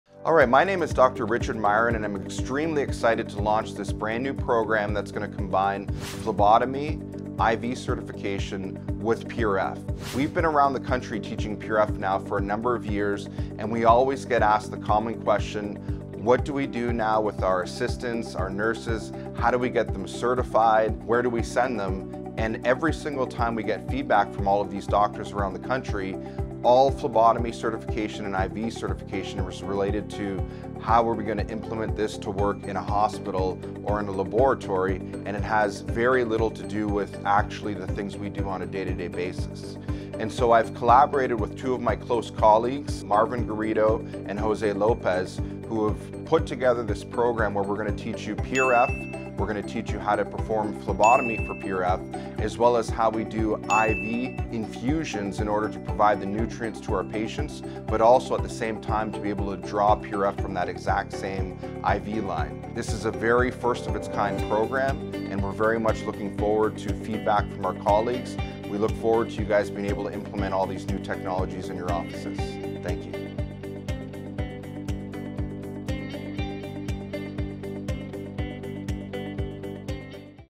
Become a Certified National Phlebotomist! | NEW Online PRF Phlebotomy Program
Calling all clinicians! Join Dr. Richard Miron in revolutionizing your practice with our brand-new online course, PRF Phlebotomy National Certification Program. 🩸 Dive into the fusion of phlebotomy techniques, IV infusion, and PRF, guided by esteemed instructors Jose Lopez and Marvin Garrido.

This program is tailored to meet the growing global interest in phlebotomy, with a special focus on PRF (Platelet-Rich Fibrin) production for both dental and medical settings. Our training videos are designed to help assistants, nurses, and phlebotomists understand how to optimize PRF production effectively and based on the latest evidence.